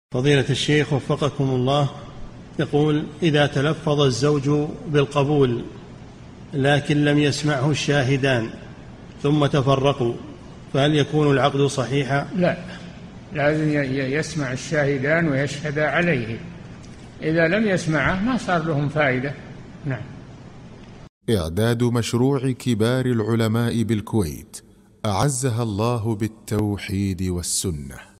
[2227 -3022] إذا تلفظ الزوج بالقبول، ولم يسمعه الشاهدان، ثم تفرقوا، فهل يكون العقد صحيحا؟
-"[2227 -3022] السؤال: إذا تلفظ الزوج بالقبول، لكن لم يسمعه الشاهدان، ثم تفرقوا، فهل يكون العقد صحيحا؟
الجَوَابُ: لا، لازم يسمع الشاهدان ويشهدا عليه، إذا لم يسمعا ما صار لهم فائدة.
للاشتراك في قناة الشيخ صالح الفوزان

للاستفسار والإقتراحات والدعم
رئيس الفريق
لمشروع كبار العلماء